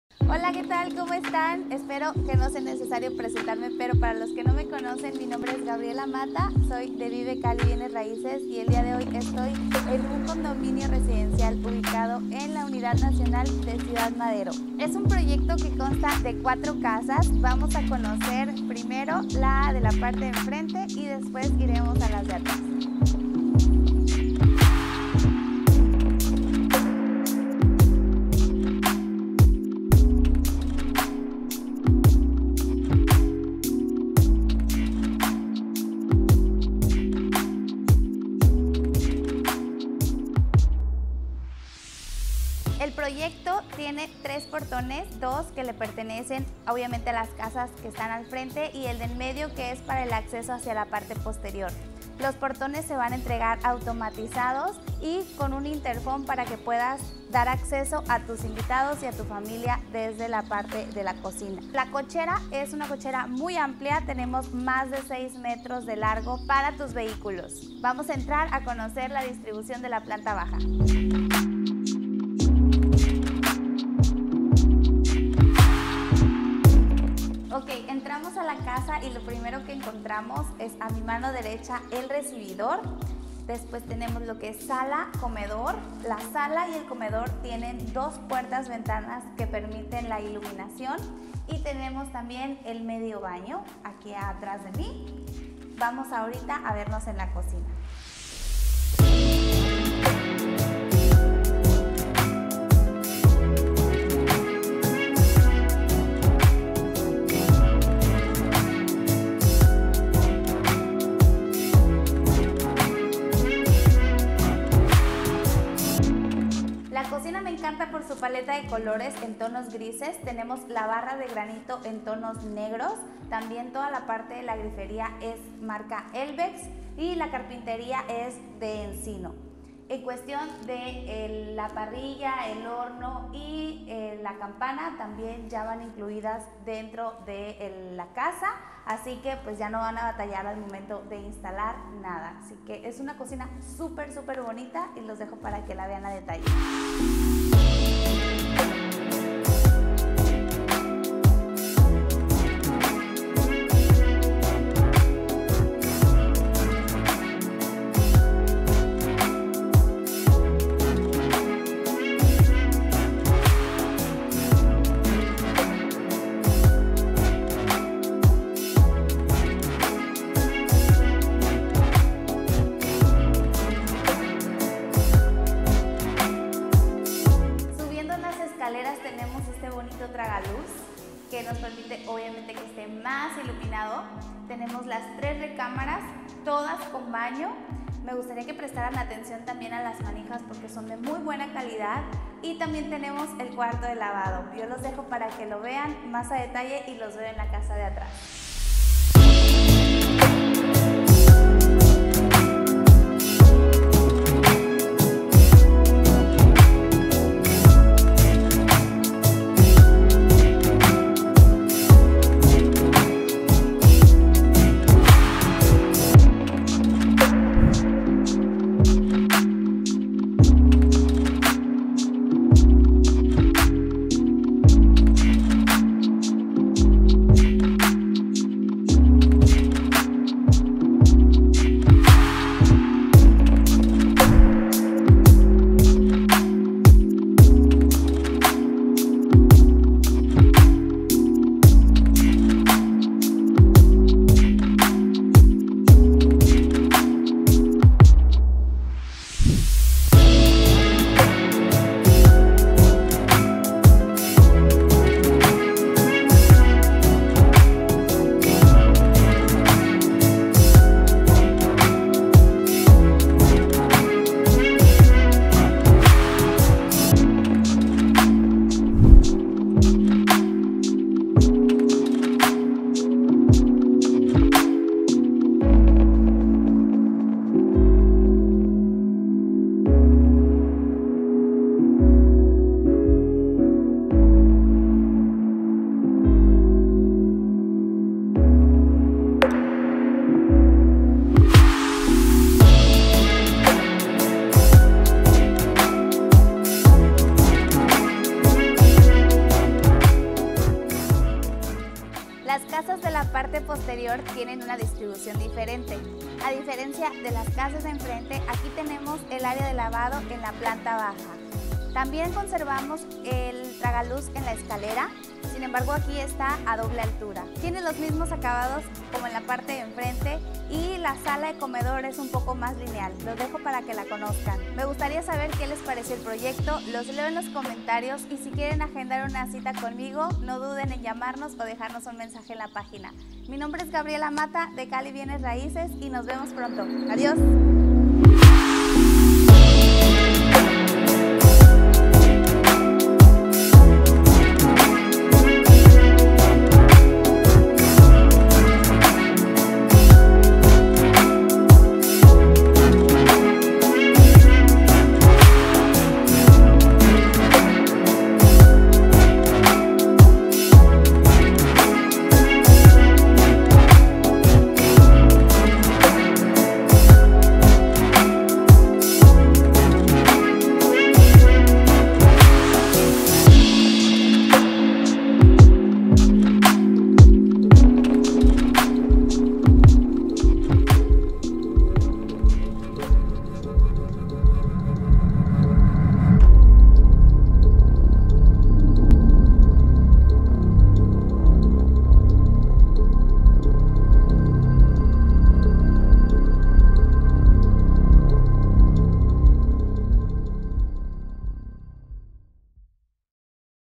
Conjunto Residencial en Unidad Nacional, ¡$3,750,000 cerca de principales Avenidas, Ciudad Madero!
Te invitamos a conocer este conjunto residencial ubicado a pocas calles de Av. Madero, en la Col. Unidad Nacional.

Las propiedades cuentan con bonitos acabados minimalistas, áreas verdes, baños en todas sus recámaras y una muy amplia cochera, además de encontrarse rodeada de centros comerciales, gimnasios, restaurantes y a 10 minutos de la playa Miramar.

VK029 Características:
200m² de Terreno
231m² de Construcción
Valor de la propiedad: $3,750,000mxn

Más información:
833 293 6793   I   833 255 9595 I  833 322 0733